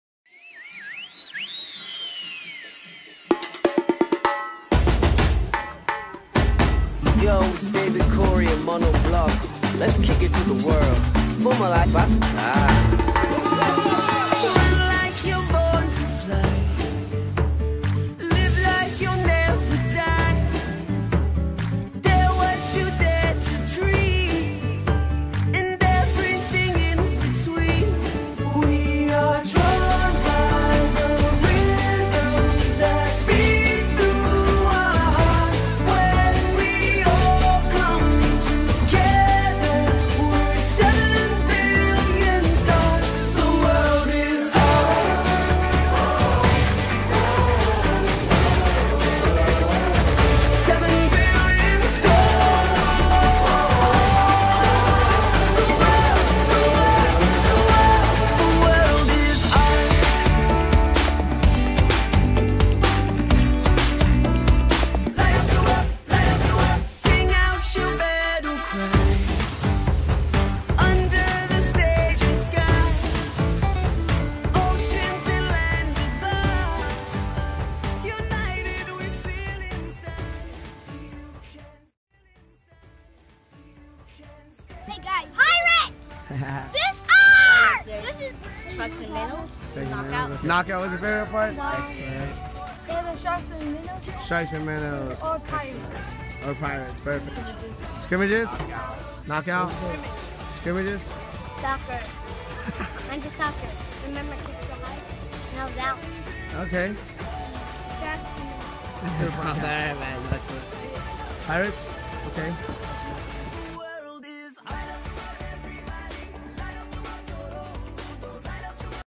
Soccer Academy Club Toluca Summer Soccer Camp 2014
This Summer SACTA launched its very first summer camp in its history! Here is a glimpse of how thins went...